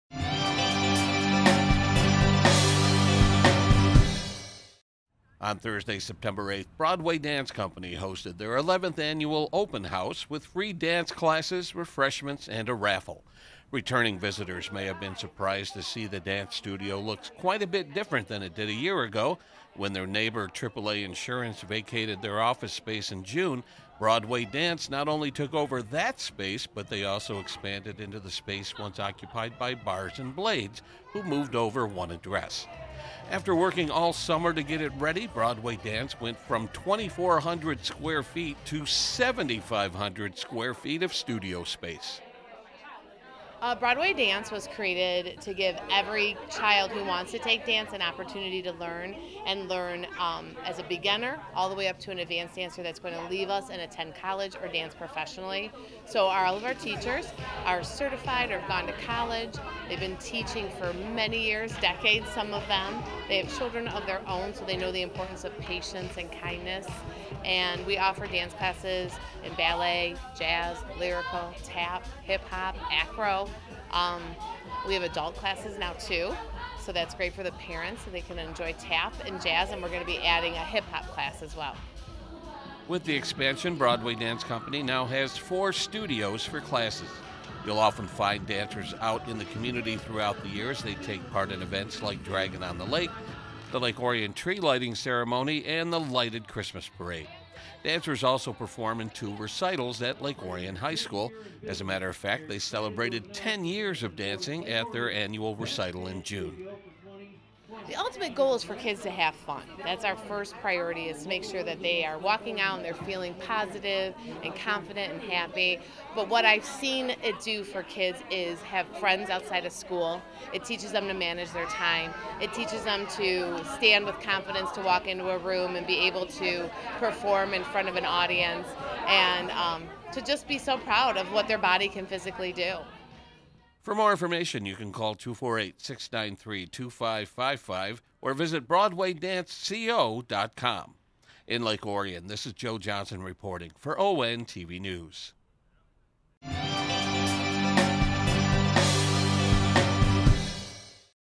ONTV News Break: Broadway Dance Co. Open House
On Thursday, September 8, 2022, Broadway Dance Co. hosted their 11th annual Open House and showed off their newly expanded studio space.